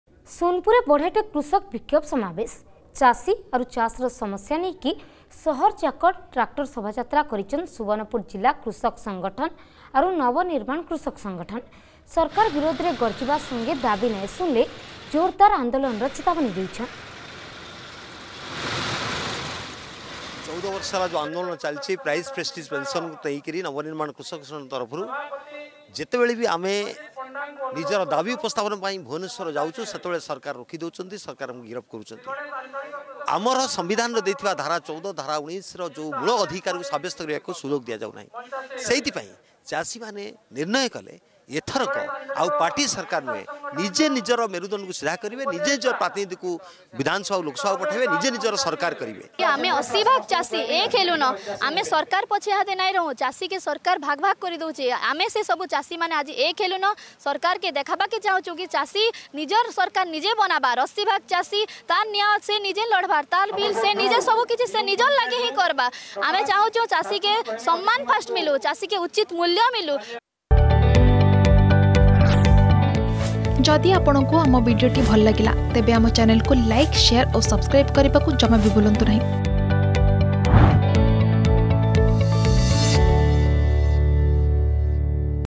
ସୋନପୁରେ ବଢେଟେ କୃଷକ ବିକ୍ଷୋଭ୍ ସମାବେଶ || Sonepur Farmers Protest
ସୋନପୁରେ ବଢେଟେ କୃଷକ ବିକ୍ଷୋଭ୍ ସମାବେଶ । ଚାଷୀ ଆରୁ ଚାଷ୍‌ର ସମସ୍ୟା ନେଇକି ସହର ଯାକର୍ ଟ୍ରାକ୍ଟର ଶୋଭାଯାତ୍ରା କରିଛନ୍  ସୁବର୍ଣ୍ଣପୁର ଜିଲ୍ଲା କୃଷକ ସଙ୍ଗଠନ ଓ ନବ ନିର୍ମାଣ କୃଷକ ସଙ୍ଗଠନ ।